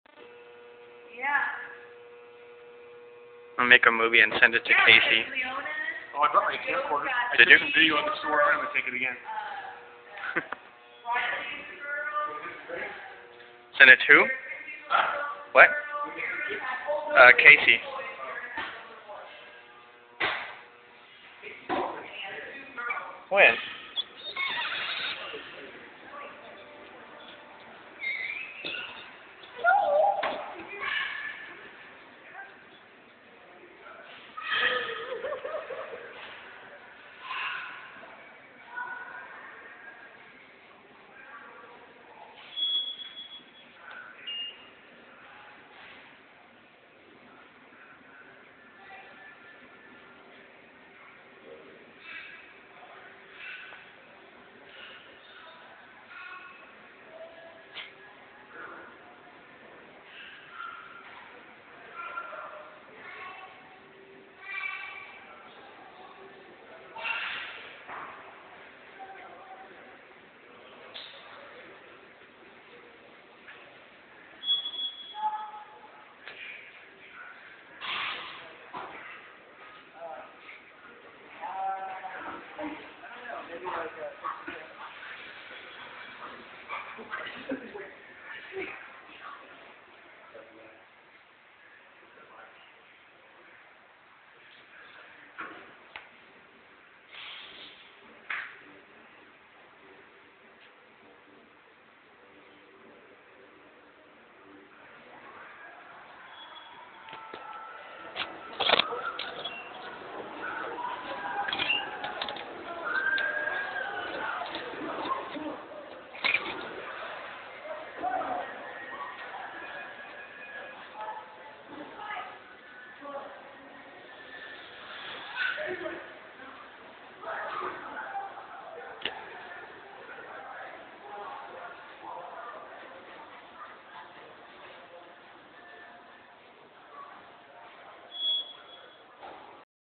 CompUSA - Almost Empty
CompUSA in Fresno, CA 2 weeks before closing